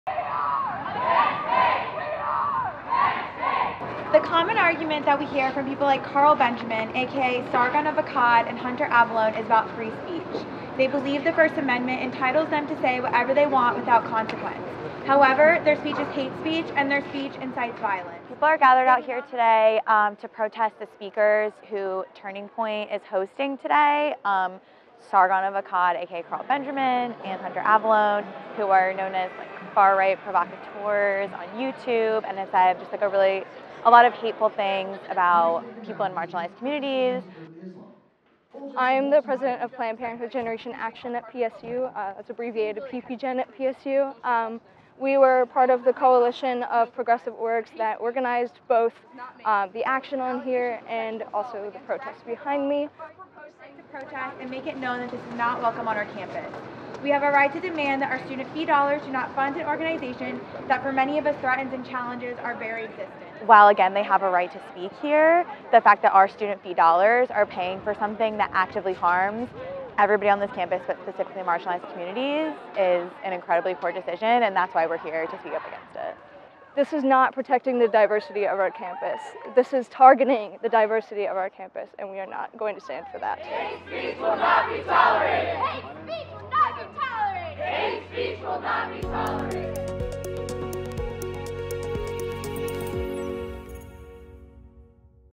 Protests break out ahead of Penn State Turning Point USA's 'The Censored'
Follow the Collegian for everything Penn State.

By Ben McClary | The Daily Collegian